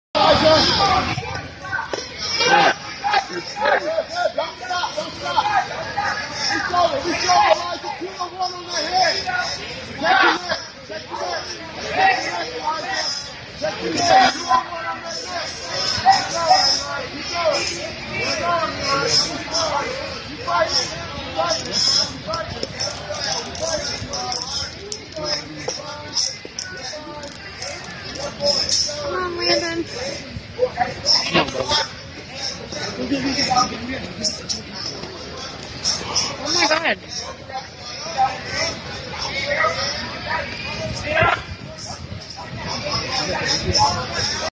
Guillotine Choke puts kid to sleep
Landon Kelley~
Newbreed Jiu Jitsu Tournament
129.9 lbs division

Kid goes to sleep for way too long after being trapped in a guillotine choke!!!